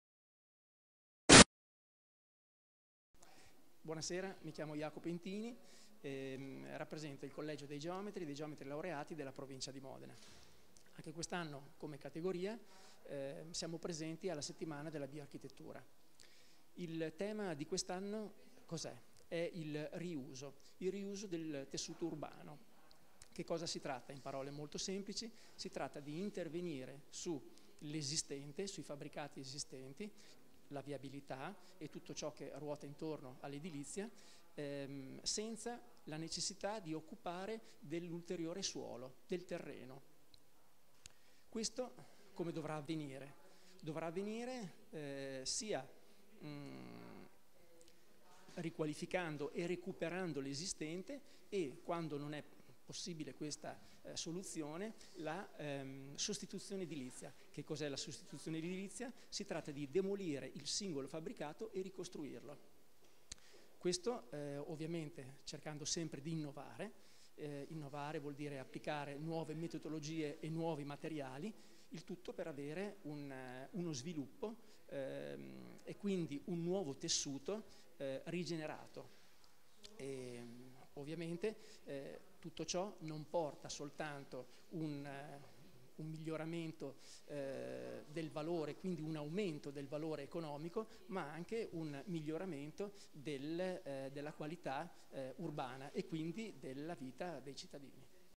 Settimana della bioarchitettura e della domotica 2015 - Jacopo Intini
Collegio Geometri e Geometri Laureati della Provincia di Modena
Parla del riuso degli ambienti preesistenti, evitando così nuovi sprechi. Ciò avviene riqualificando l'esistente e quando non è possibile si attua la sostituzione edilizia.